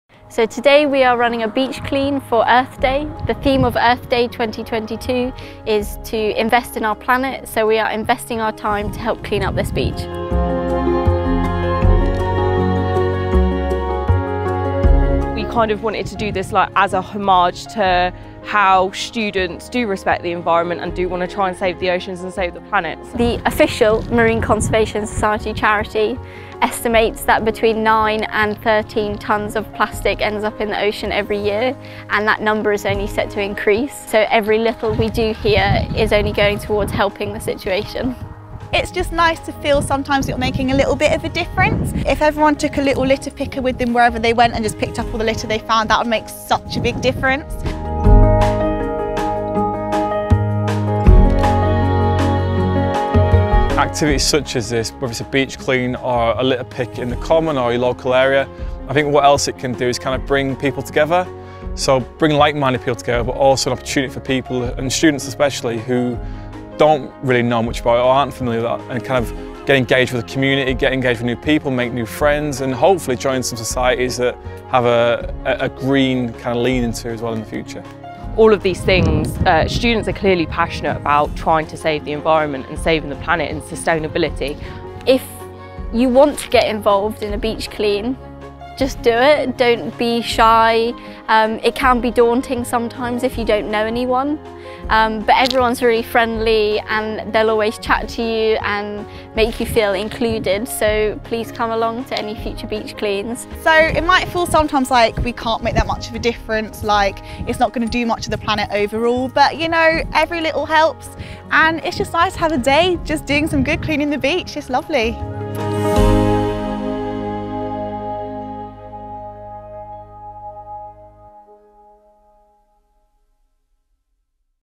Earth Day 2022 - Beach Clean
On Earth Day 2022, RAG and the Marine Conservation Society joined with SUSU to help clean up a local beach as part of SUSU's Sustainability Fortnight.